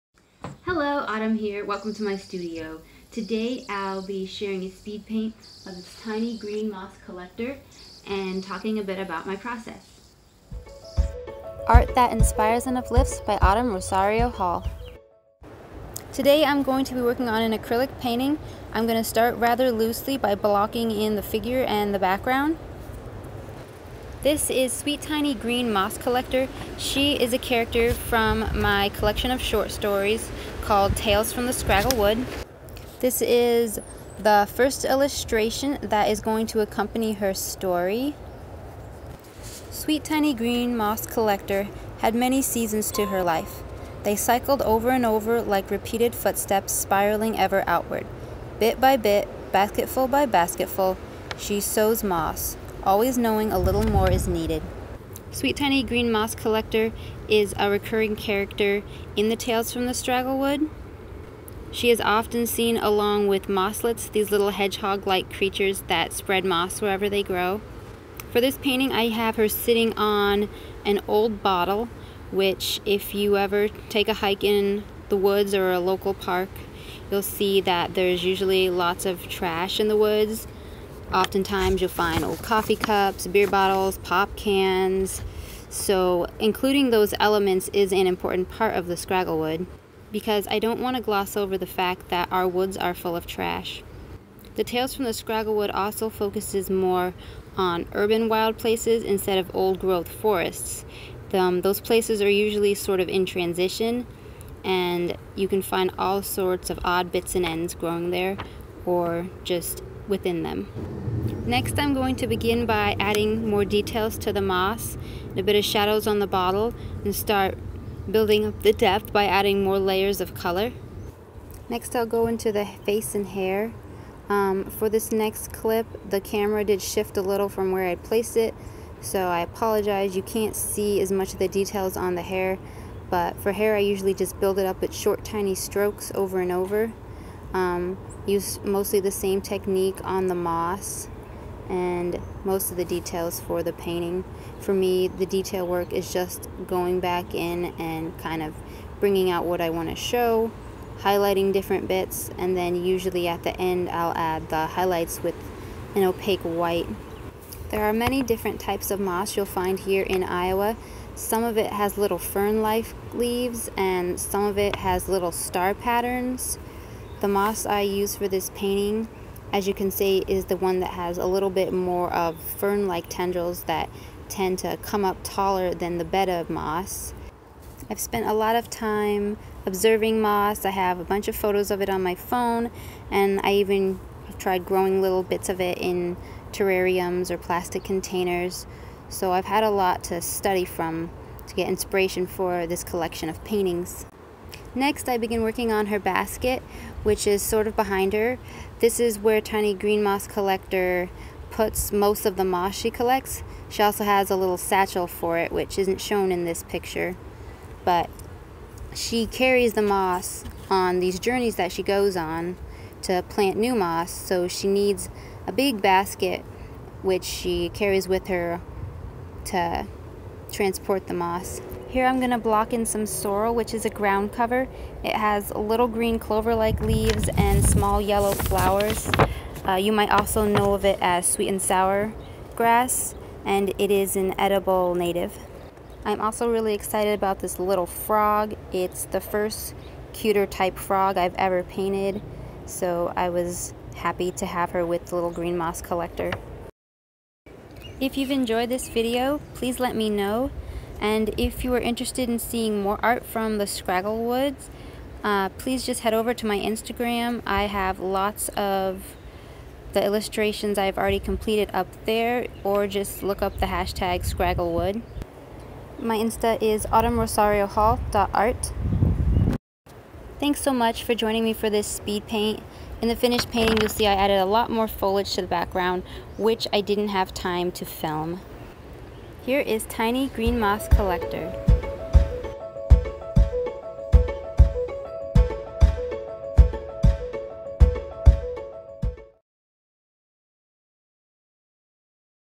Speed Painting and Process / Moss Painting / cottage core art
Learn a little about my originalcharacter Tiny Green Moss Collector and my painting process.

follow scragglewood to see more art.